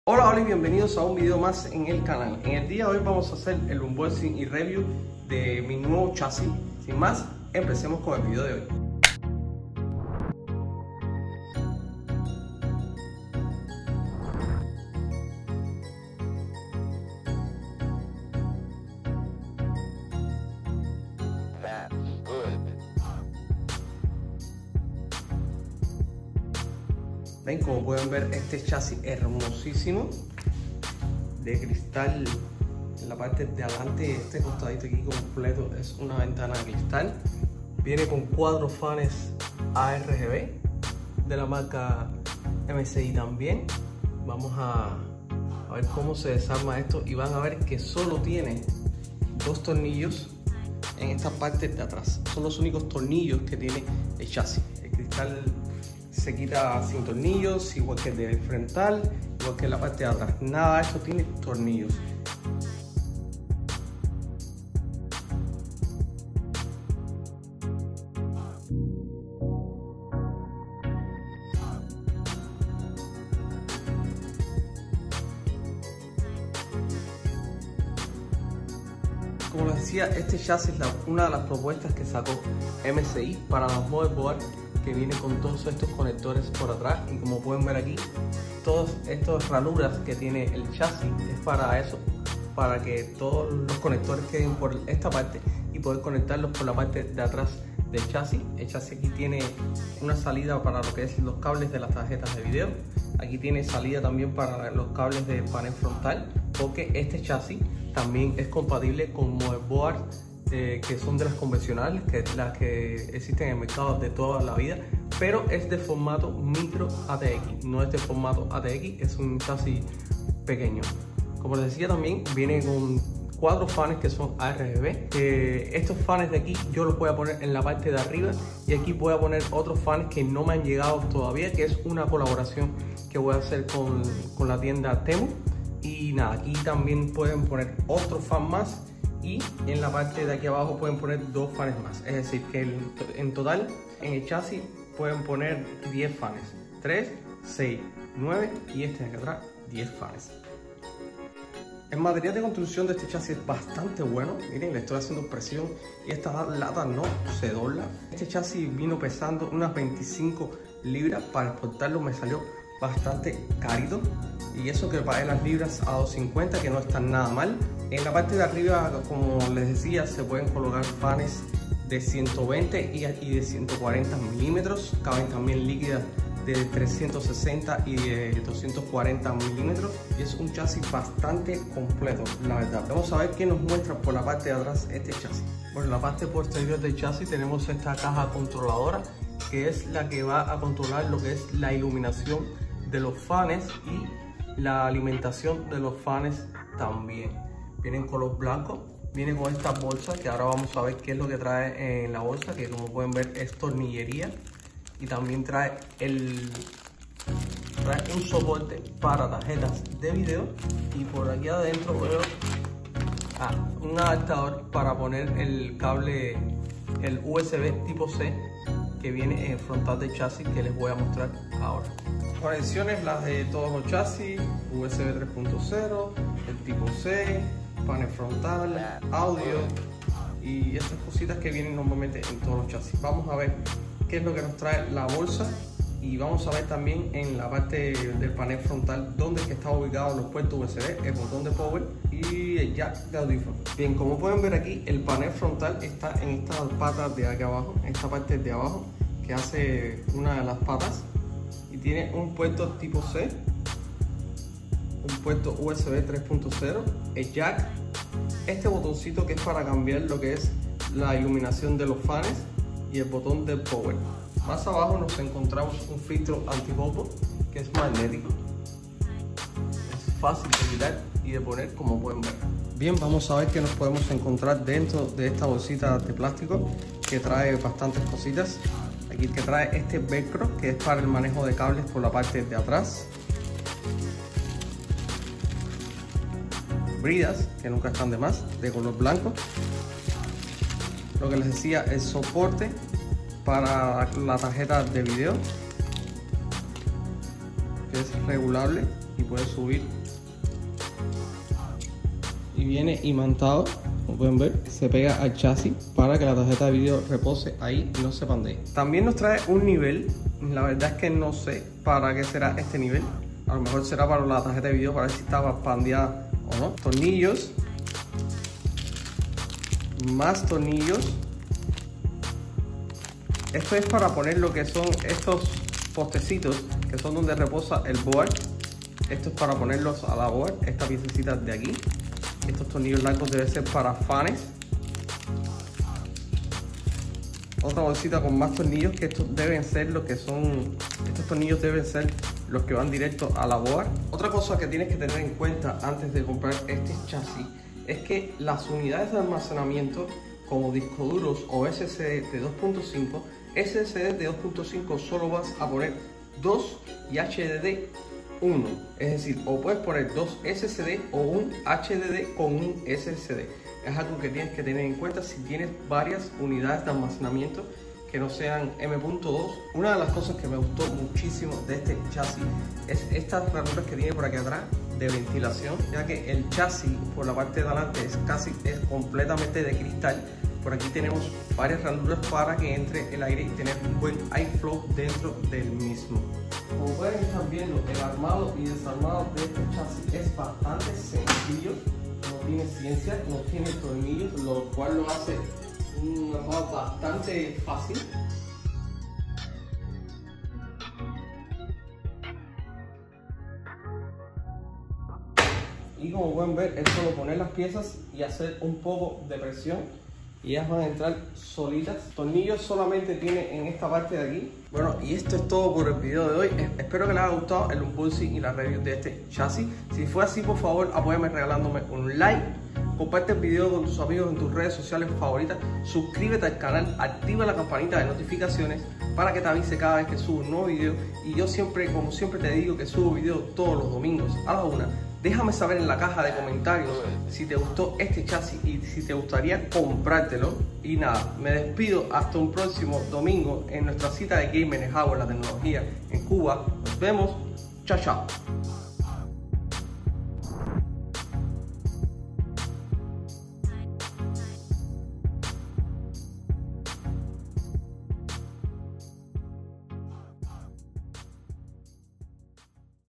MSI MAG PANO M100R PZ REVIEW EN ESPAÑOL#PROJECT ZERO#.
HOLA PIKETE LES DEJO AKI LA REVIEW EN ESPAÑOL DE ESTE GENIAL CHASIS

LINK DE AMAZON ESTADOS UNIDOS
CHASIS MSI MAG PANO M100R PZ

MISI PROJETCT ZERO#